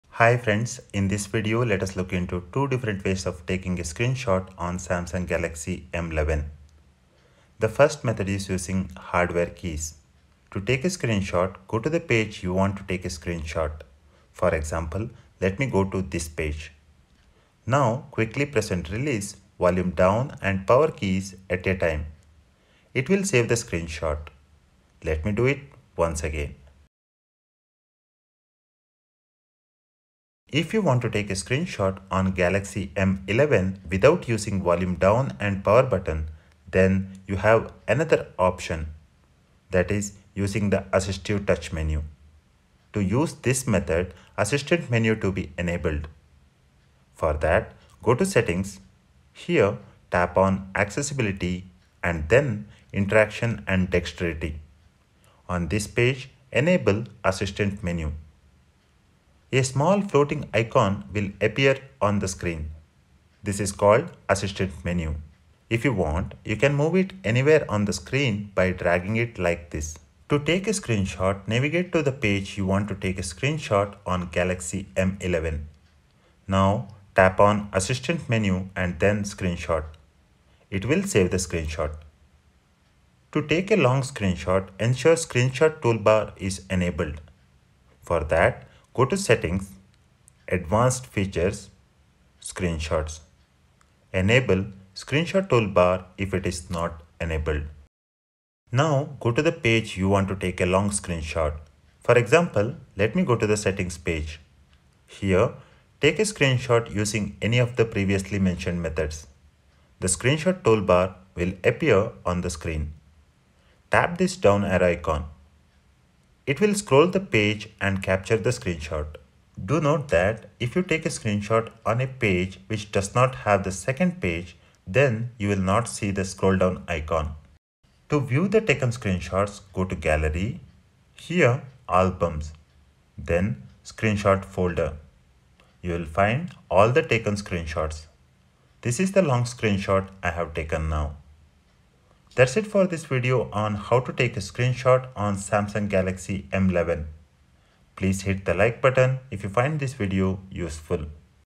How to take screenshot on Samsung Galaxy M11 - 2 Ways Plus Long screenshot
This video covers two different ways to take screenshot on Galaxy M11. It also covers taking a long screenshot or scroll screenshot on Samsung Galaxy M11.
*Timestamp*
Introduction: 0:00
Hardware Key Method: 0:08
Screenshot button method: 0:31
Long screenshot: 1:23